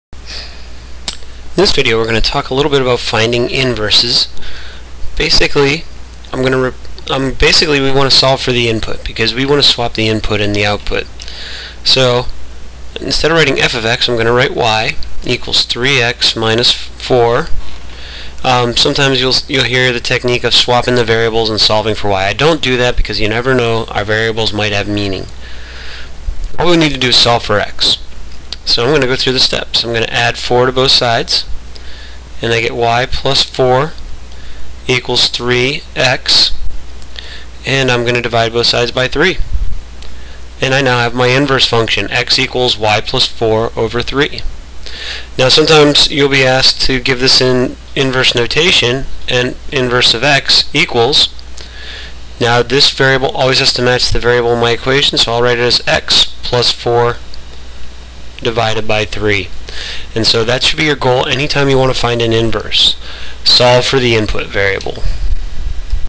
Find the Inverse of a Linear Function
In this video we find the inverse of a linear function.